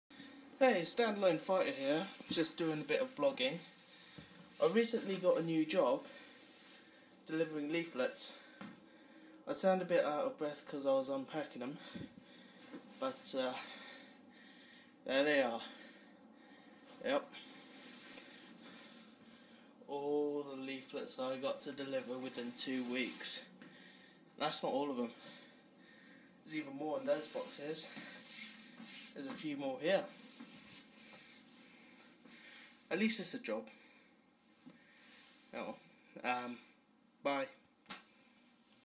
80,000 leaflets
i just got a new job:D i have to deliver over 80,000 leaflets! lol. gave me a masive shock when it all came in the post:P.

i get paid £20 for every 1,000 leaflets i deliver. if i do them all, i get £1,600:)